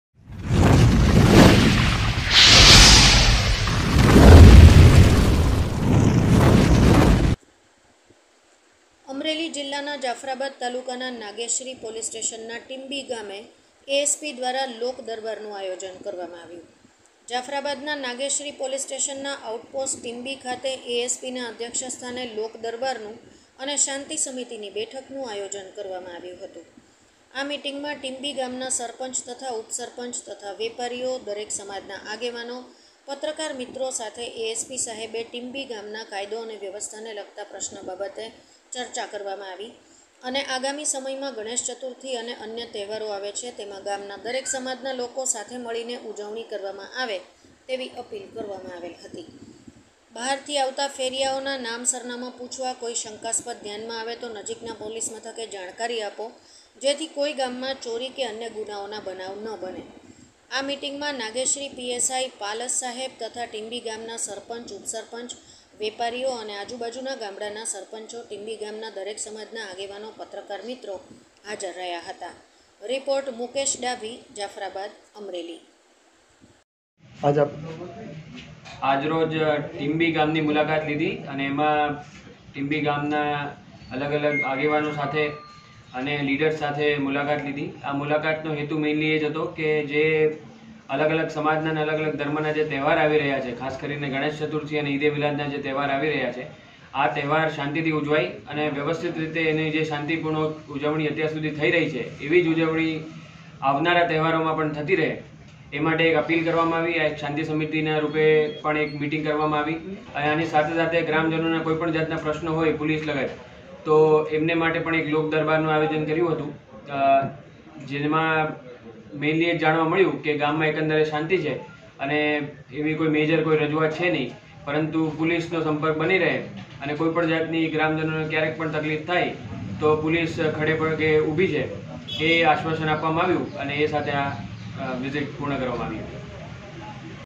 NEWS અમરેલી જીલ્લાના જાફરાબાદ ના નાગેશ્રી પોલીસ સ્ટેશનના ટીંબી ગામે ASP દ્વારા લોક દરબારનું આયોજન
અમરેલી જીલ્લાના જાફરાબાદ તાલુકાના નાગેશ્રી પોલીસ સ્ટેશનના ટીંબી ગામે ASP દ્વારા લોક દરબારનું આયોજન કરવામાં આવ્યુ.

જાફરાબાદના નાગેશ્રી પોલીસ સ્ટેશનના આઉટ પોસ્ટ ટીંબી ખાતે   ASPના અધ્યક્ષ સ્થાને લોક દરબારનું અને શાંતિ સમિતિની બેઠક નું આયોજન કરવામાં આવ્યુ હતુ

આ મીટીંગ માં ટીમ્બી ગામના સરપંચ તથા ઉપસરપંચ તથા વેપારીઓ  દરેક સમાજ ના આગેવાનો  પત્રકાર મિત્રો સાથે A. S. P. સાહેબે ટીમ્બી ગામના કાયદો વ્યવસ્થાને લગતા પ્રશ્ન બાબતે સર્ચા કરવામાં આવી અને આગામી સમયમાં ગણેશ ચતુર્થી અને અન્ય તહેવારો આવે છે તેમાં ગામના દરેક  સમાજ ના લોકો સાથે મળીને ઉજવણી કરવામાં આવે તેવી અપીલ કરવામાં આવેલ હતી

તેમજ બહારથી આવતા ફેરિયાઓના નામ સરનામાં પુછવા અને કોઈ શંકાસ્પદ ધ્યાનમાં  આવે તો નજીકનાં  પોલીસ મથકે જાણકારી આપો જેથી કોઈ ગામમાં ચોરી કે અન્ય ગુન્હાઓના  બનાવ ન બને

આ મીટીંગમાંનાગેશ્રી psi. પાલસ સાહેબ તથા  ટીંબી ગામના સરપંચ તથા ઉપસરપંચ તથા વેપારીઓ અને આજુબાજુ ના ગામડા ના સરપંચો  ટીંબી ગામના   દરેક સમાજના આગેવાનો પત્રકાર મિત્રો હાજર રહ્યા હતા

રિપોર્ટ. મુકેશ ડાભી. જાફરાબાદ. અમરેલી